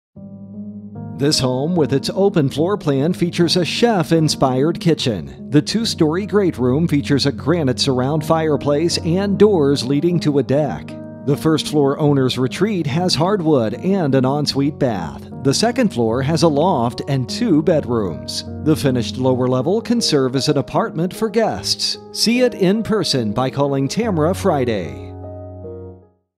3289 Long Meadow Drive Allison Park, PA | ColdwellBankerHomes.com
3289 Long Meadow Drive, Allison Park, PA

Open Floor Plan with a chef inspired kitchen thoughtfully designed by Cuvee Kitchens; includes high end appliances, granite and painted Medallion cabinetry. The two story great room is light filled with a granite surround gas fireplace with doors leading to the private Trex deck with retractable awn